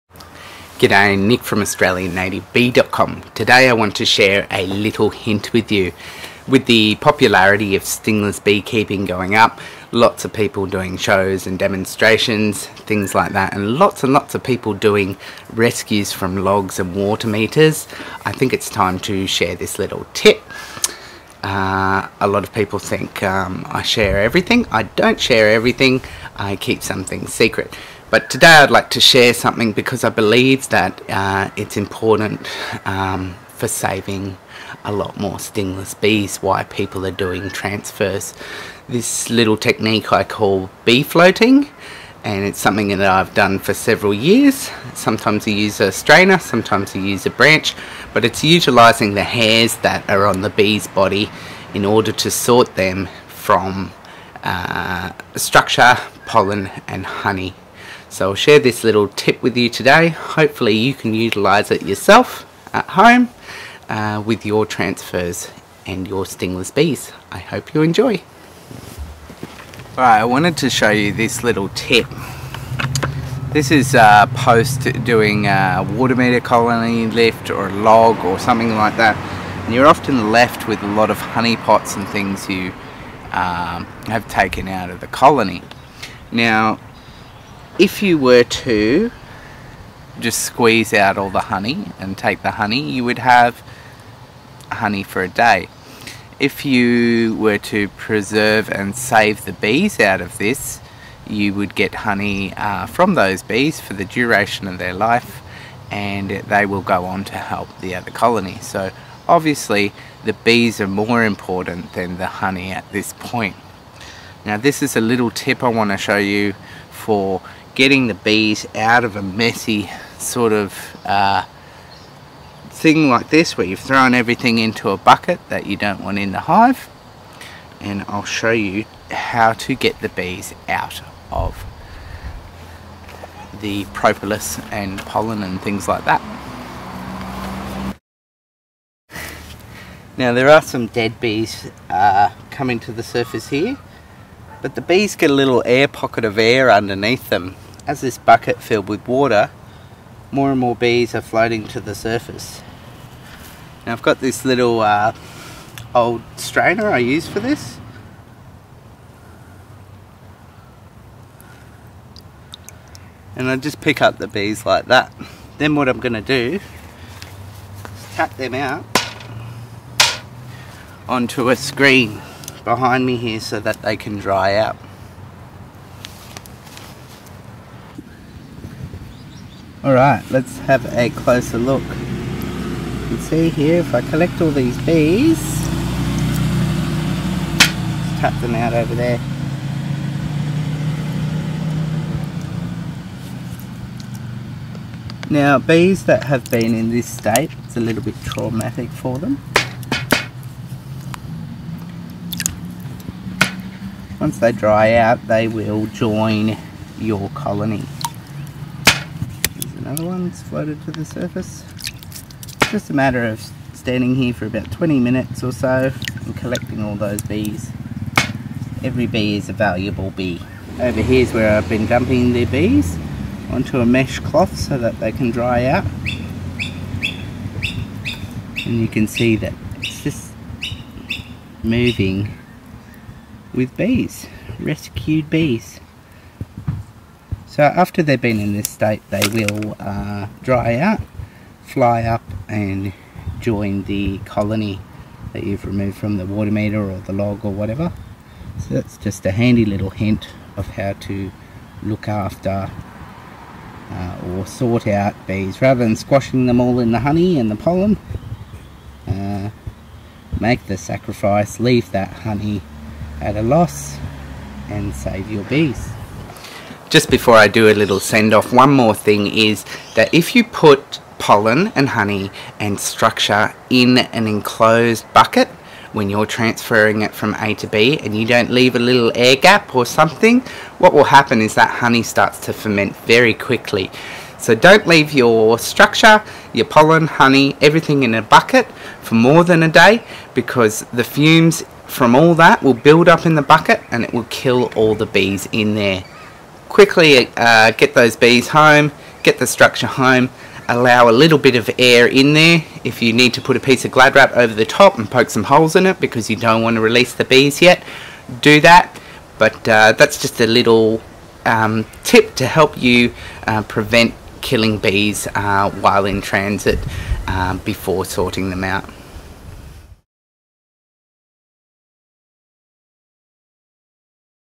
Bee Floating- Stingless bees 🐝
A little tip for sorting bees from a messy rescue removal.

Please visit

Why not take an online stingless bee keeping course